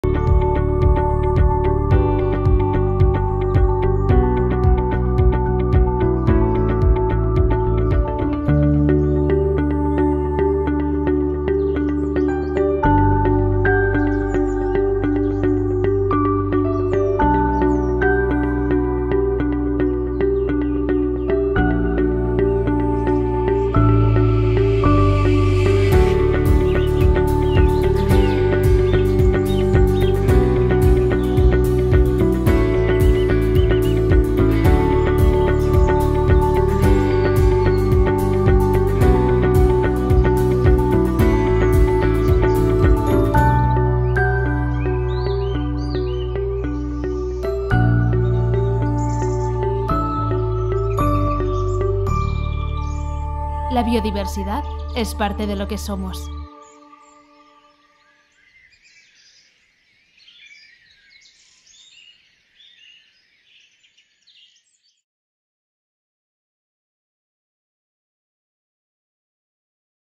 BiObserva - Voluntariado ambiental corporativo de seguimiento de aves
BiObserva es un programa de seguimiento de la biodiversidad basado en la ciencia ciudadana, impulsado por el Grupo Agbar, para el seguimiento de las aves y el control de las especies de flora invasoras en el ciclo integral del agua.